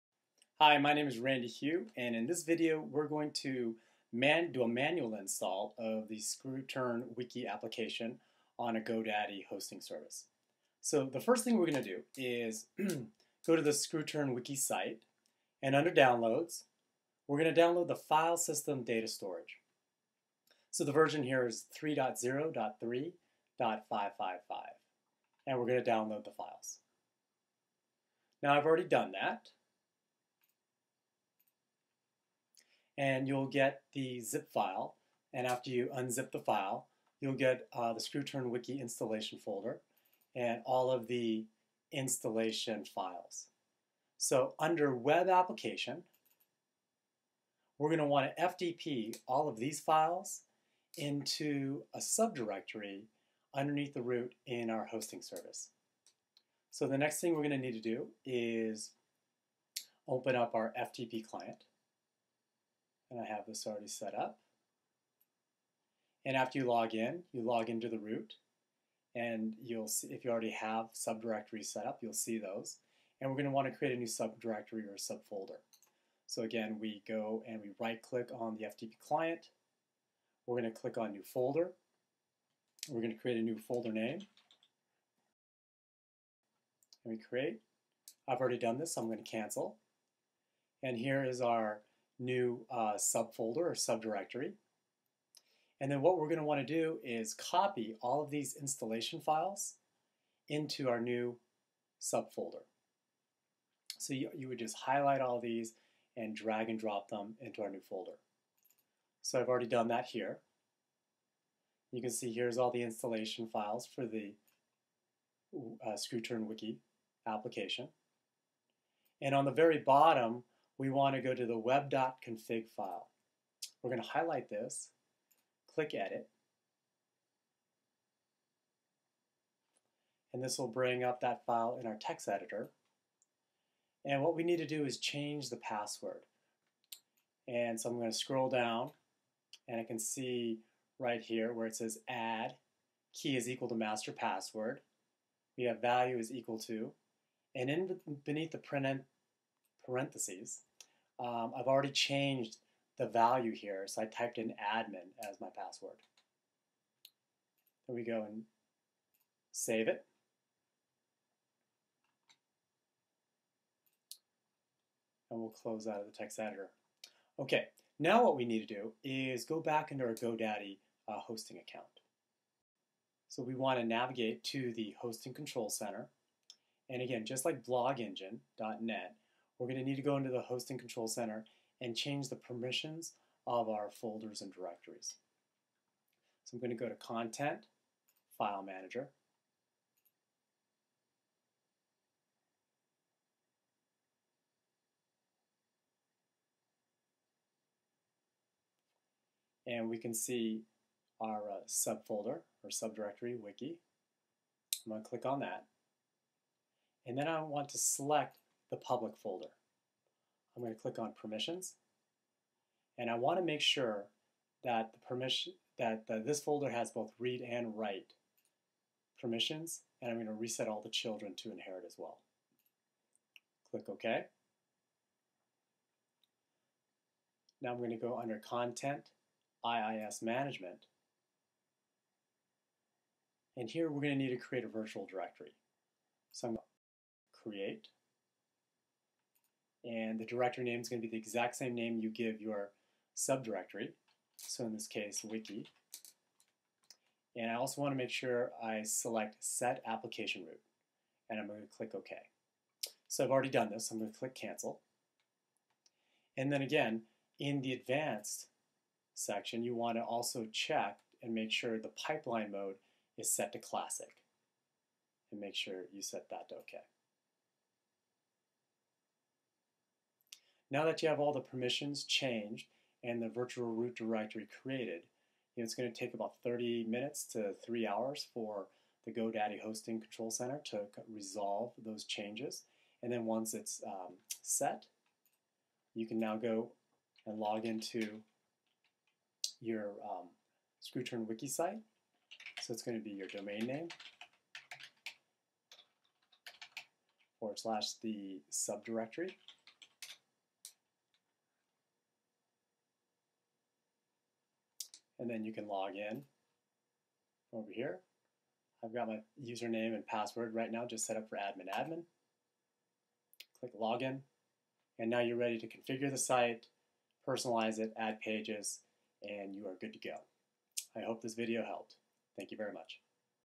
Install ScrewTurn Wiki with Godaddy Hosting Service
ScrewTurn Wiki Install Step by Step Instructions

Instructions on how to set up BlogEngine 1.6 (web) on Godaddy hosting service.

Step 1. Download ScrewTurn Wiki install files (File Data Storage)

Step 2. FTP into hosting service and create "sub directory" or "sub folder" ie wiki

Step 3. FTP all files & folders within the ScrewTurnWiki folder that you downloaded

Step 4. Open the "web.config" file with a text editor and set the MasterPassword field with a password that will be used for the built-in admin account, for example:

add key="MasterPassword" value="my_password_123" /

Step 4. Go to Hosting Control Center in Godaddy

Step 5. Go to File Manager under Content, select the public File . Select Permissions and give it Write Permissions & inherit.

Step 6. Go to IIS Control under Content, create virtual directory, same name you gave the sub directory and select "root application". Also select "advanced" and make sure pipeline mode is set to Classic.

Step 7. From your browser go to your new subdirectory, ie

If ScrewTurn Wiki doesn't come up right away, wait a little while (30 mins to 3 hours) the permission changes you made in the Hosting Control Center may still be resolving.